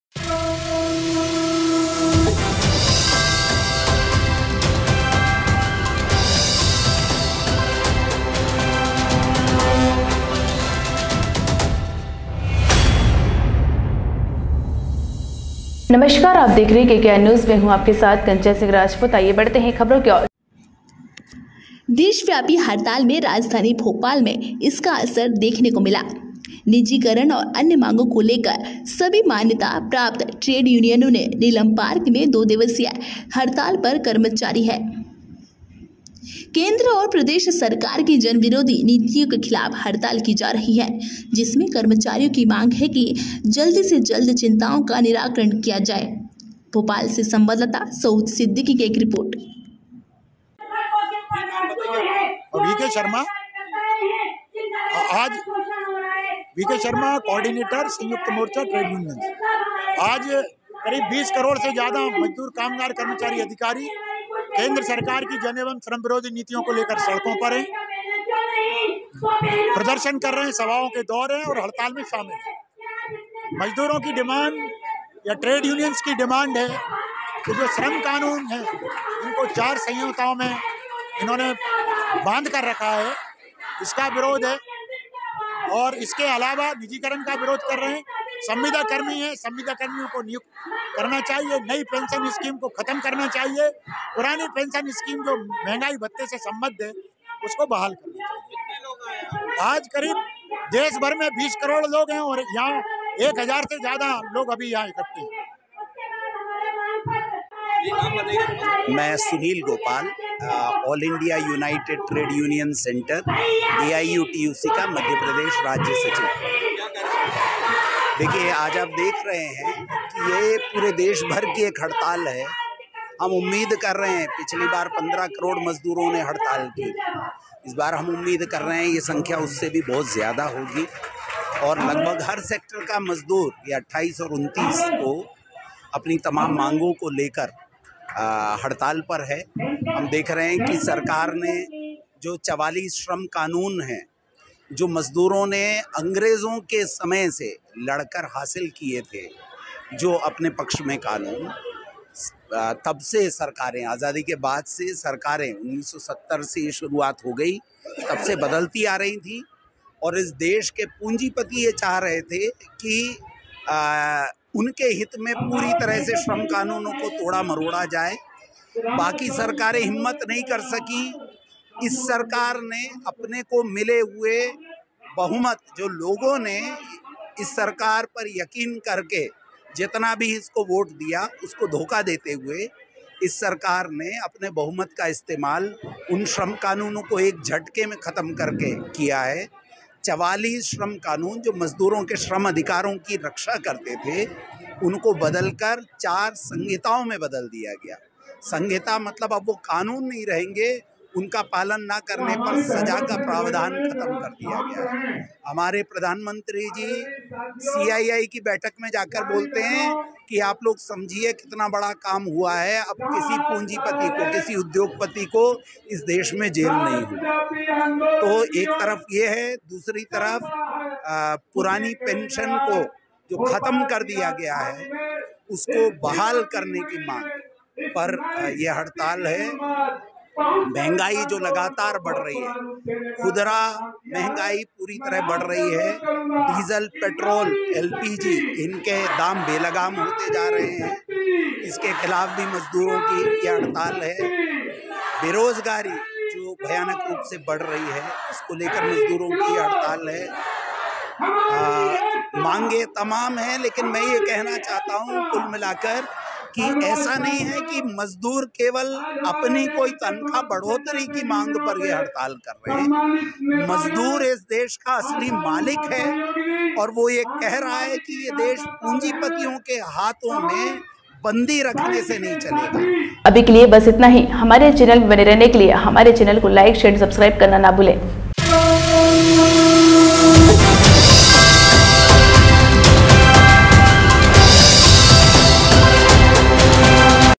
बैंक कर्मचारियों सहित अन्य विभाग के कर्मचारी भी हुए शामिल
एंकर

 देशव्यापी हड़ताल में राजधानी भोपाल में इसका असर देखने को मिला निजी करण और अन्य मांगों को लेकर  सभी मान्यता प्राप्त ट्रेड यूनियनों ने नीलम पार्क में दो दिवसीय हड़ताल पर कर्मचारी है  केंद्र और प्रदेश सरकार की जनविरोधी नीतियों के खिलाफ हड़ताल की जा रही है जिसमें कर्मचारियों की मांग है कि जल्द से जल्द चिंताओं का निराकरण किया जाए

निजी करण के विरोध में दो दिवसीय आंदोलन जारी

 केंद्र और प्रदेश की जन विरोधी नीतियों के खिलाफ सड़कों पर उतरी ट्रेड यूनियने

बाइट
बीके शर्मा ट्रेड यूनियन कोऑर्डिनेटर

 बाइट
सुनील गोपाल ऑल इंडिया यूनाइटेड ट्रेड सेंटर मध्य प्रदेश राज्य सचिव

भोपाल से संवाददाता सऊद सिद्दीकी की एक रिपोर्ट